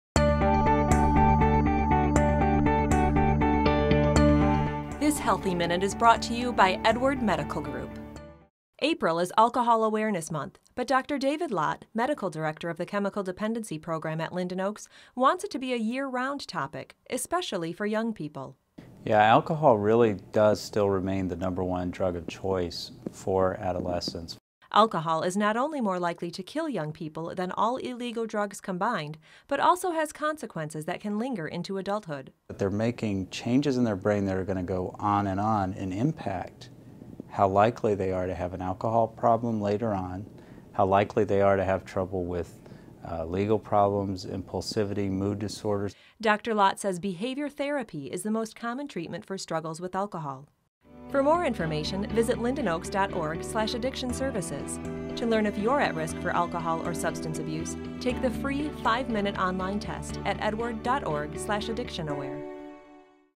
Edward Medical Group Healthy Minute: Alcohol Awareness Month
Dr. David Lott, Medical Director of the chemical dependency program at Linden Oaks, discusses the importance of alcohol awareness, especially in teens.